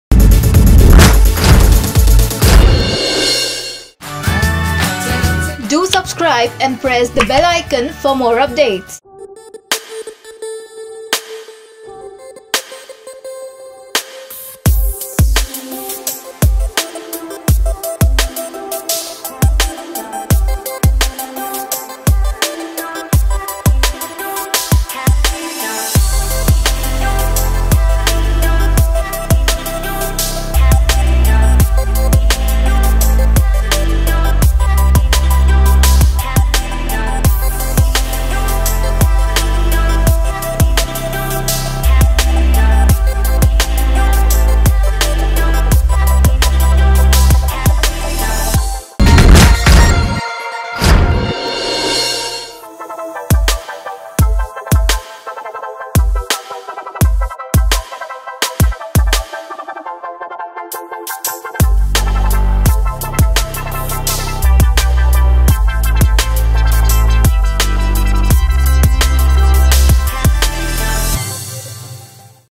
Race 3: Salman Khan REVEALS His New LOOK | Prime Bollywood | EPN
Watch the video to see Salman Khan's new look from Race 3.